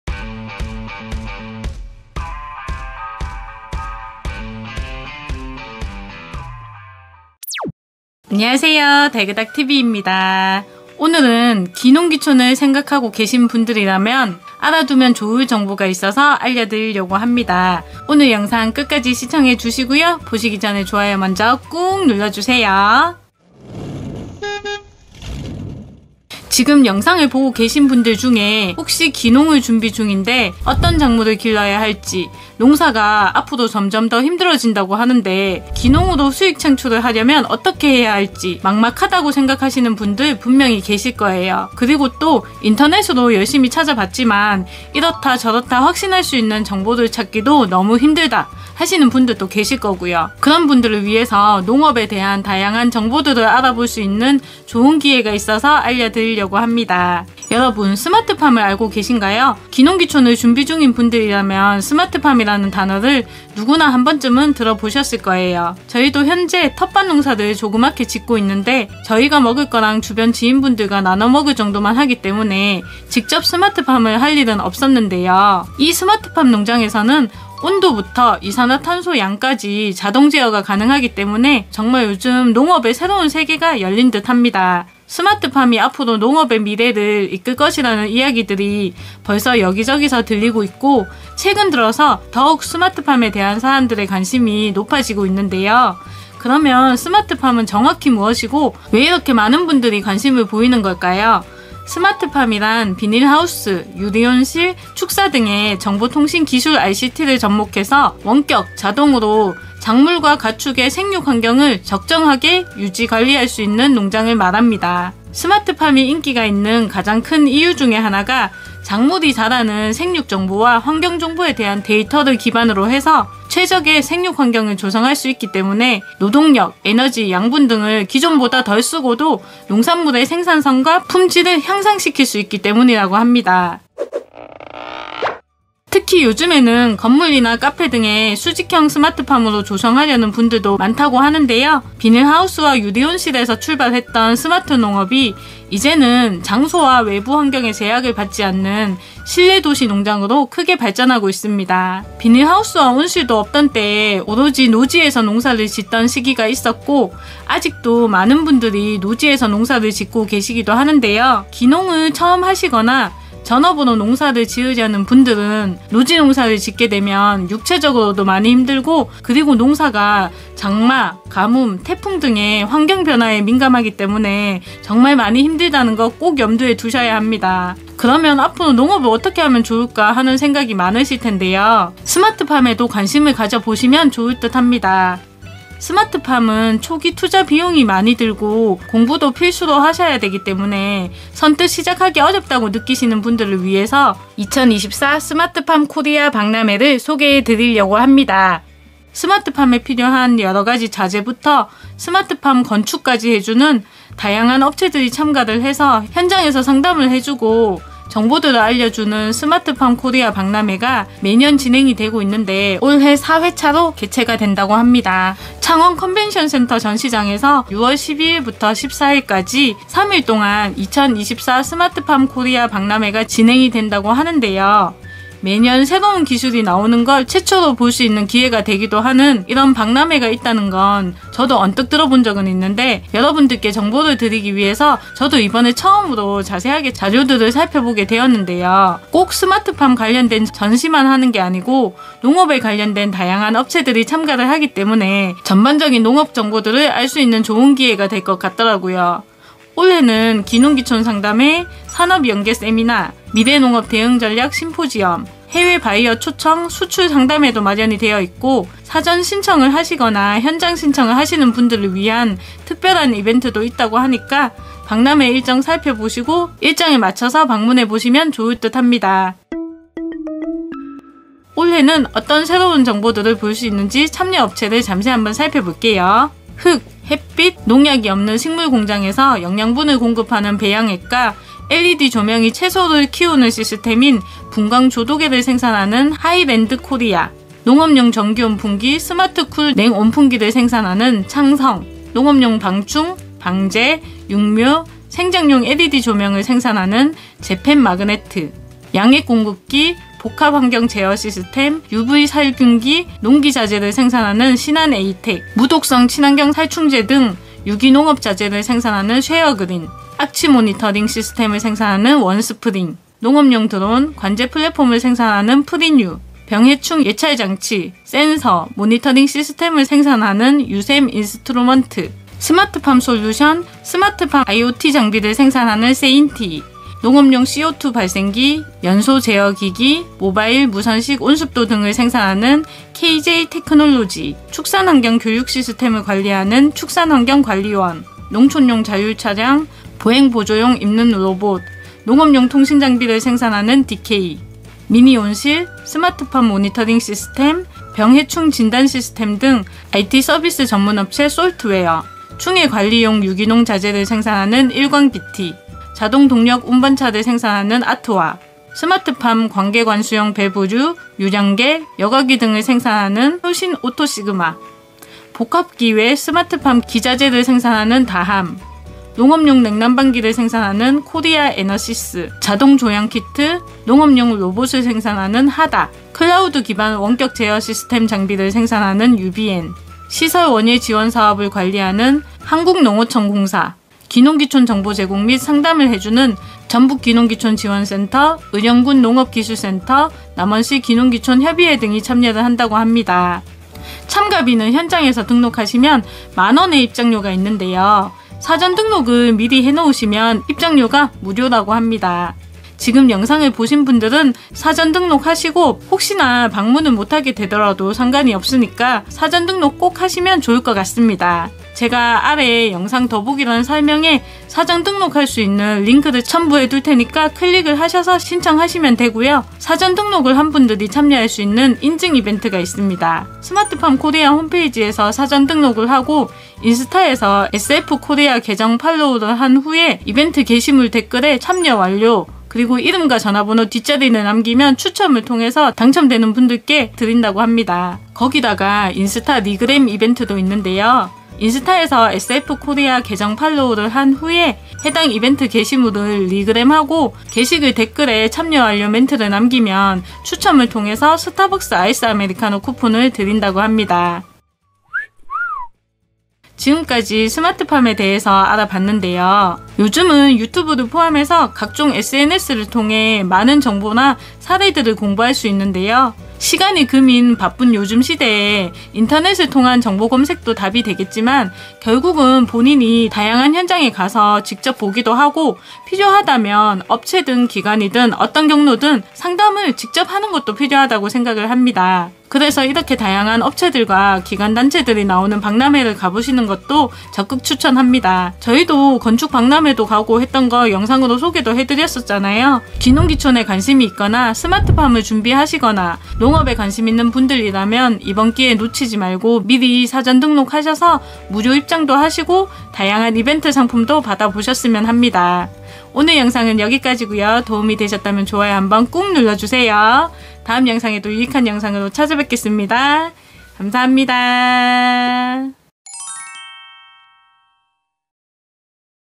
요즘 대세는 돈이 되는 스마트팜!ㅣ농업박람회에서 미리 체험하고 결정하세요!!
▶달그닥TV 소통 카카오톡 ㄴ 친구추가_ID로 추가 : ekfrmekr79(달그닥79) 로 검색하세요
▶달그닥TV 응원, 영상 제작 후원하기 : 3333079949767 카카오뱅크 임미옥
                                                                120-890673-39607 KEB하나은행 임미옥

- 행사명 : 2024 스마트팜 코리아(SFKOREA)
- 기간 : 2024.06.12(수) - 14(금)
- 장소 : 창원컨벤션센터(CECO) 1,2전시장

※ 경남 최대규모 농업박람회 스마트팜 코리아 ※
홈페이지에서 사전등록신청 하시면 현장에서 무료로 입장 가능합니다.

효과출처 - 편집하는여자/VREW/셀바이뮤직/비디오몬스터/VITA/미리캔버스/캡컷/키네마스터/블로